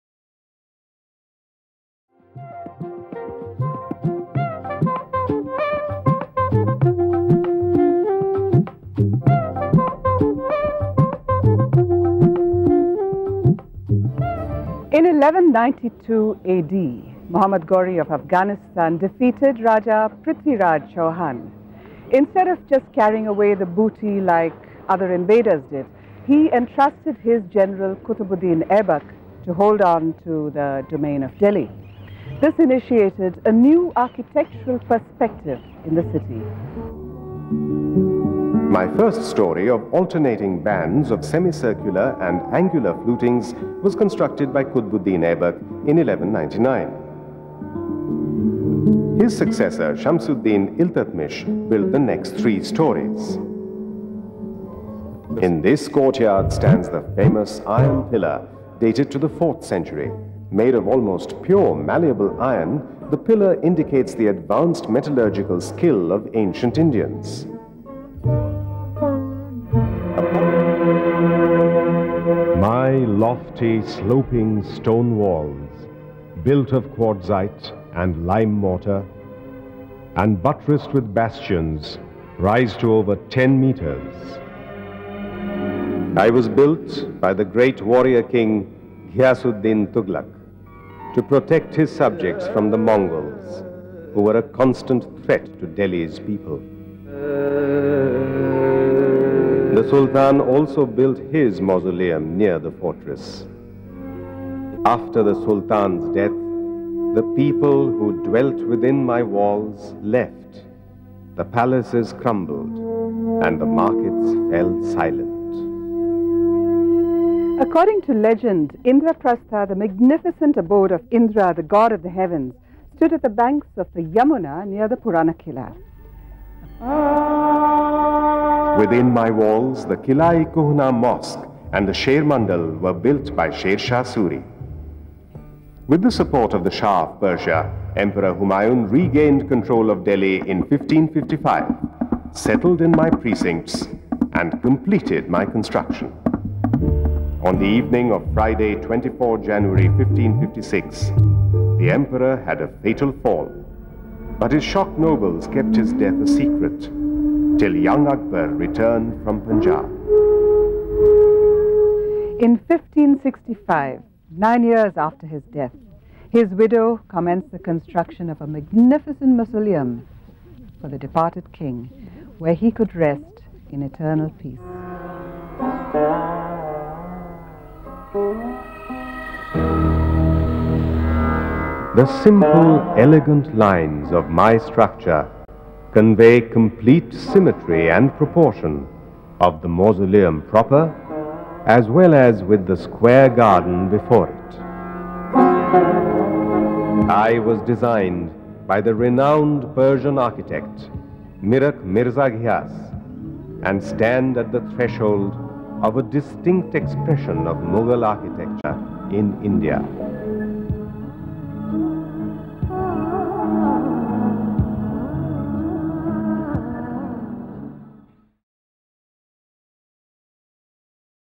Echoes from the Past: Old Monuments of Delhi
The architecture of Delhi has witnessed an incredible diversity- starting with the intricate designs from the Mughal era to the modernist style seen in Lutyen's Delhi. 'Echoes from the Past' delves into the past of Delhi as manifested in the old monuments that survive in the city today. Monumental structures like the Qutub, Tughlaqabad Fort, Purana Quila, Humayun's Tomb, Safdarjung Tomb and the Red Fort resonate with tales from a bygone era- often narrated in the epics like the Mahabharata, when the city was called Indraprastha. Today Delhi bustles with activity as the capital city of India and amidst the numerous areas, these monuments keep alive the history of India.